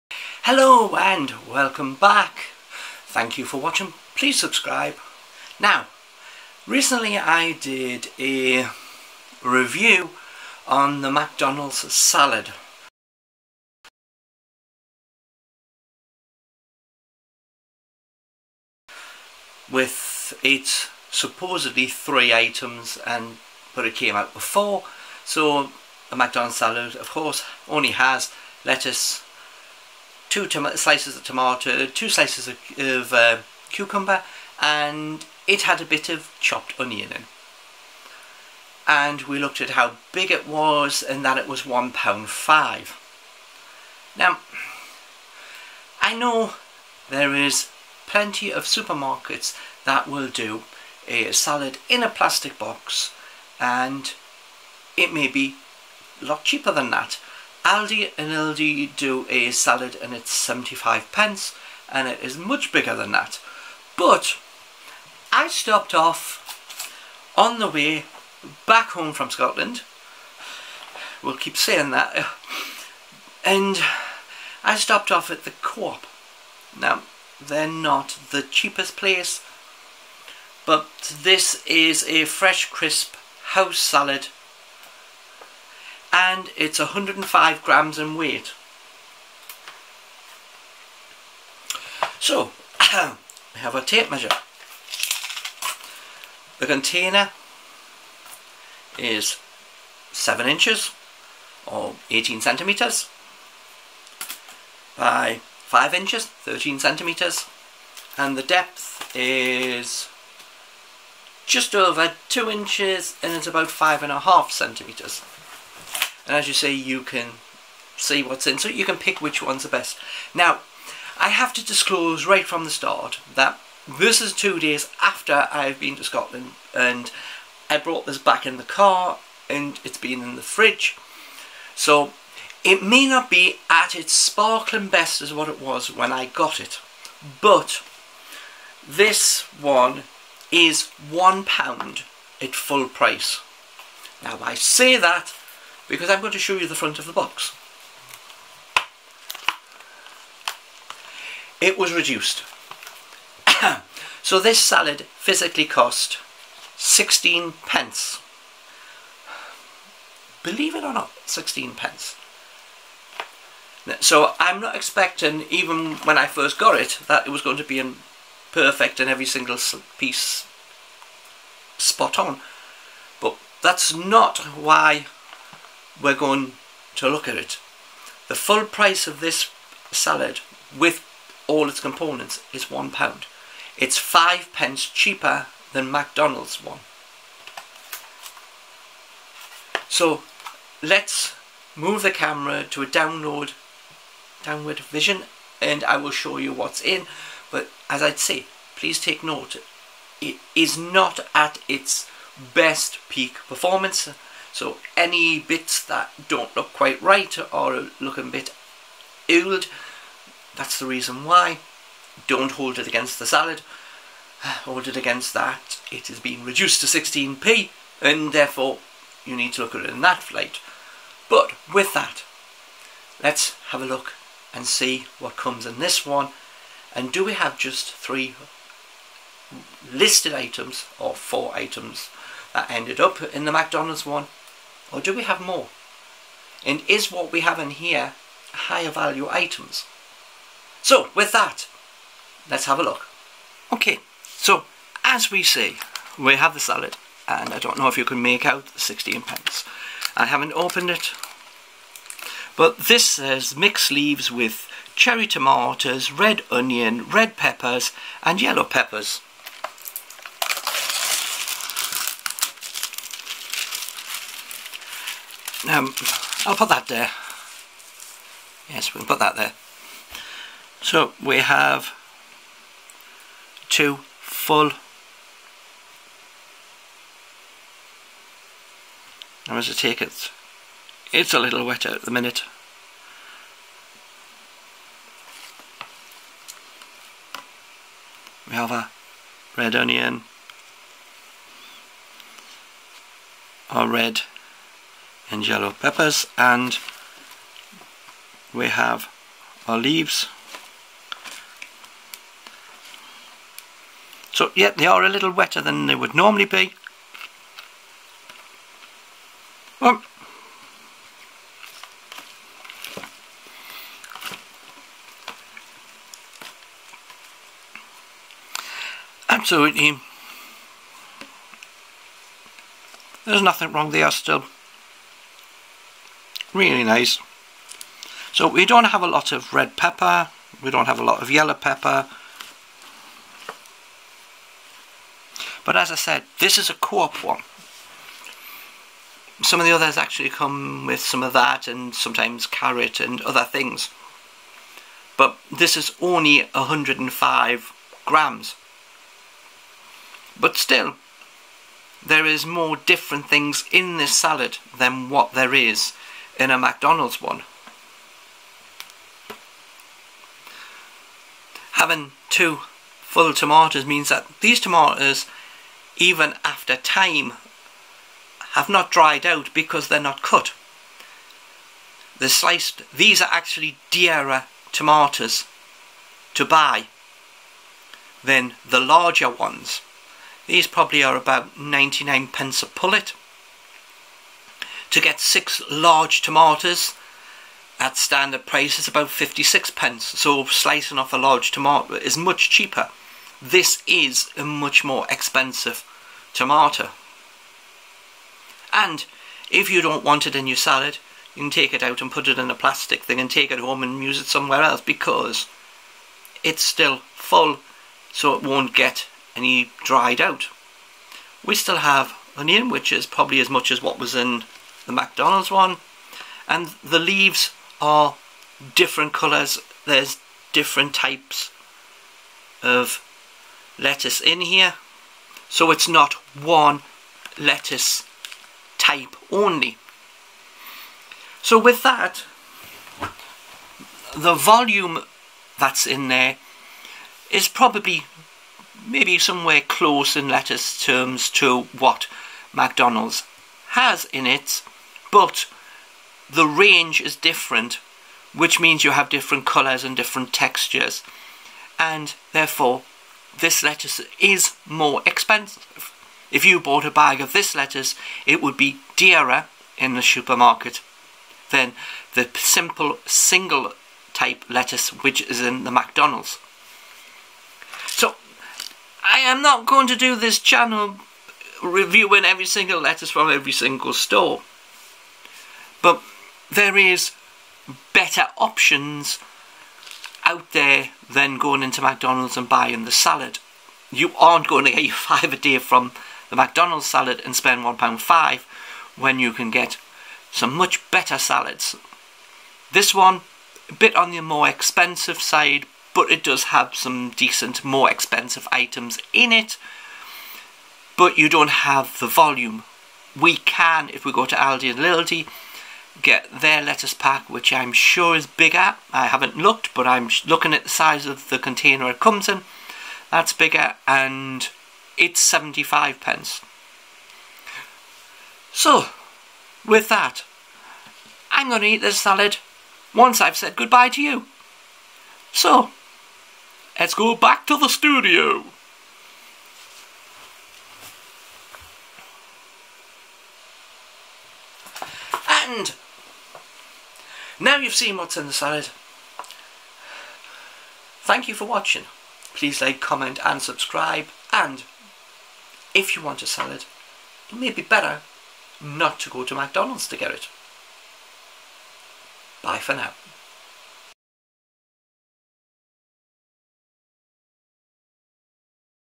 Coop Salad vs McDonalds Salad
I got a Coop (A UK supermarket chain)  Salad for at full price £1 less than a Salad gotten from McDonalds recently and video is up showing what you get in that.   We compare what the Coop says make a salad VS McDonalds say makes one